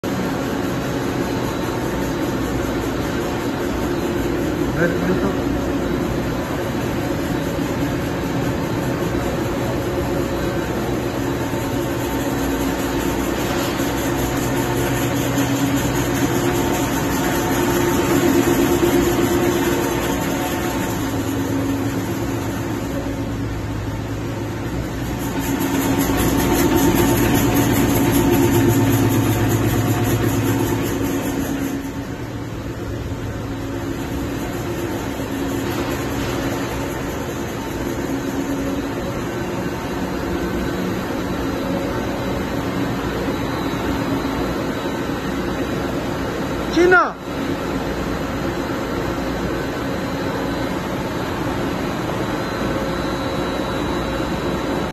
6 Colors Gearless CI flexo printing machine for paper
6 colors, 1300mm printing width, 300m/min speed, 400-800 printing repeat length. It's for paper cup, paper bag printing.